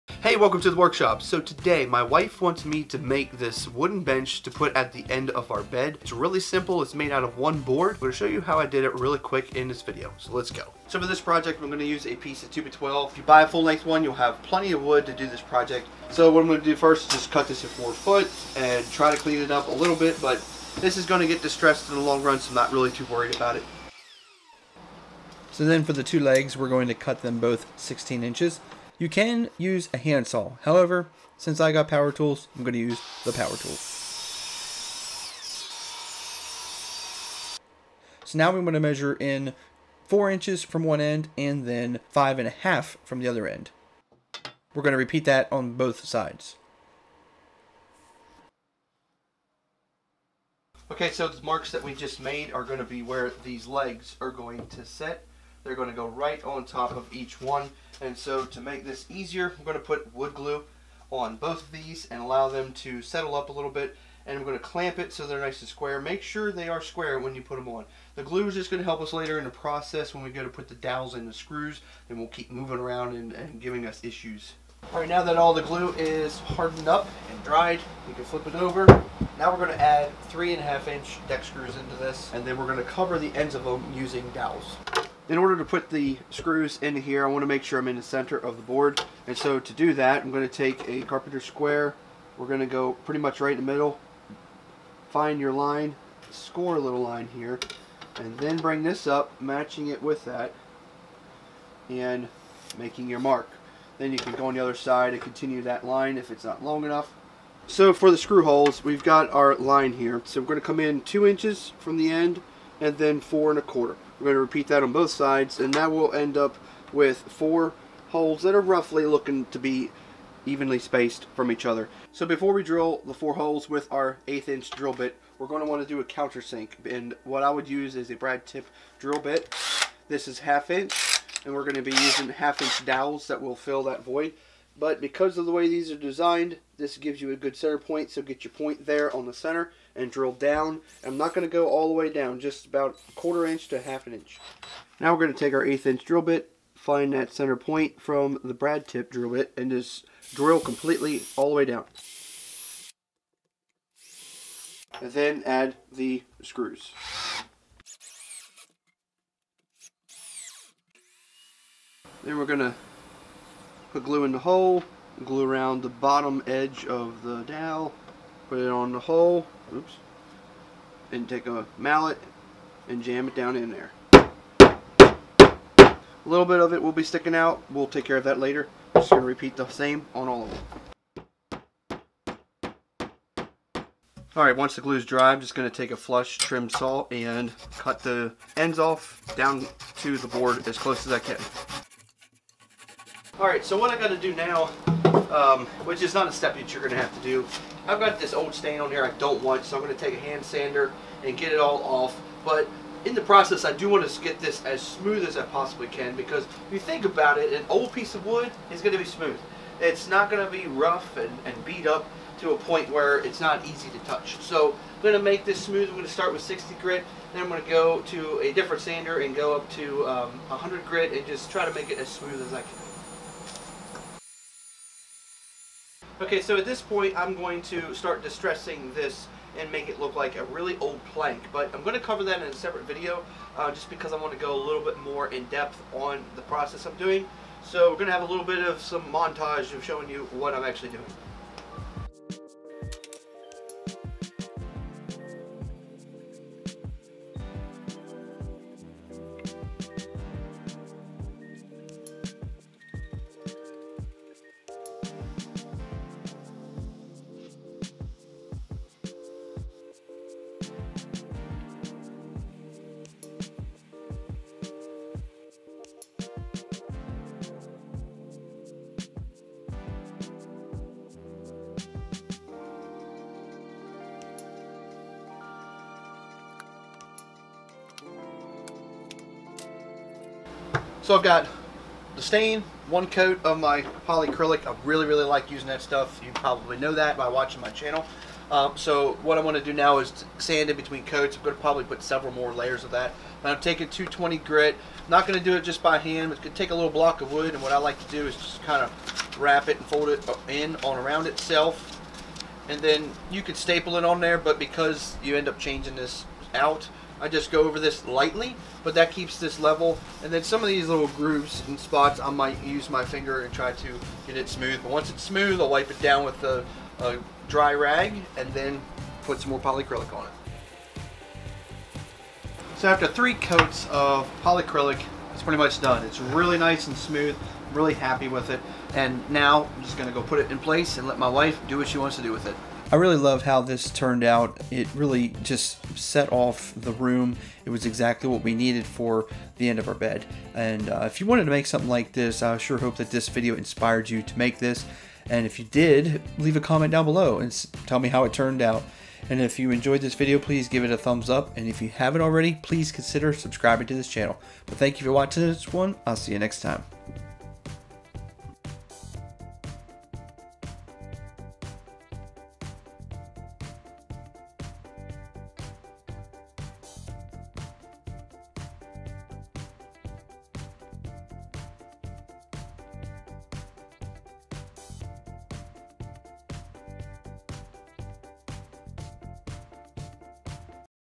How to Make a Basic Bench
How to make a basic bench. In this video, I used some of these tools to make this project. You will also need a 8ft board of 2x12 to make this bench.

Glue Bottle
Flush Trim Saw
Relitec Squares
Brad tip Bits
Drill Bits

I do DIY, Tips and project videos and I have more videos like this coming soon!